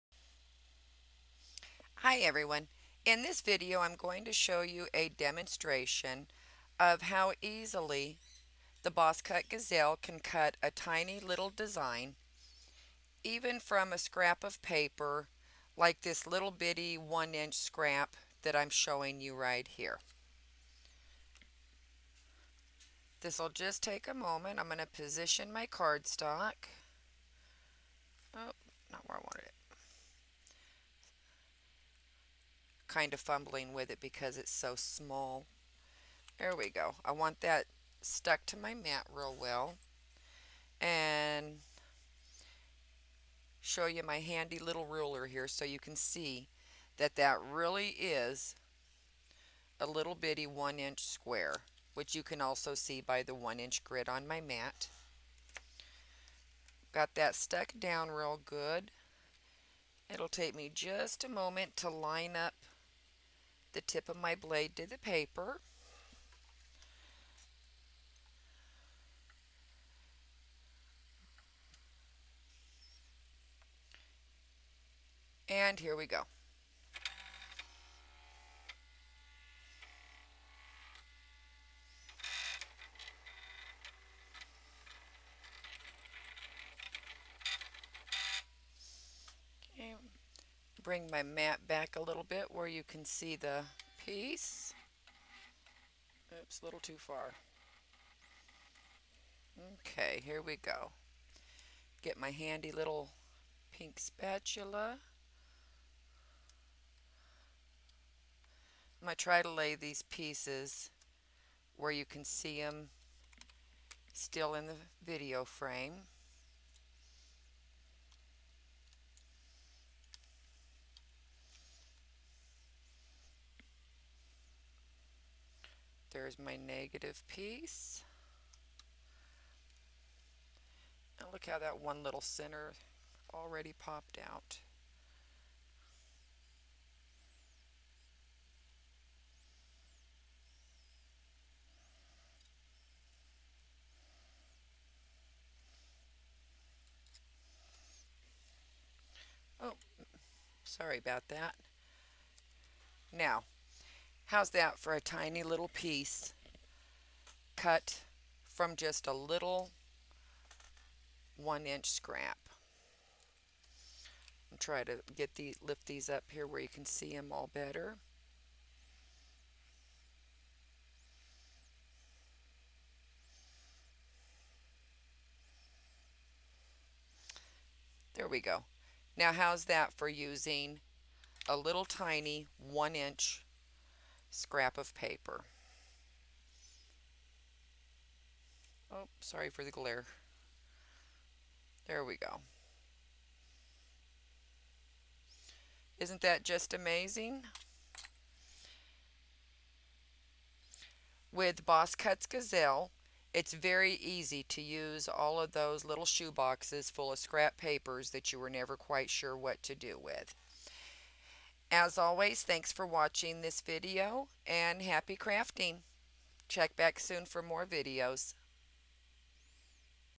Boss Kut's Gazelle cuts even the tiniest scraps!
Demonstration of the Boss Kut Gazelle cutting a tiny design from a 1 inch scrap!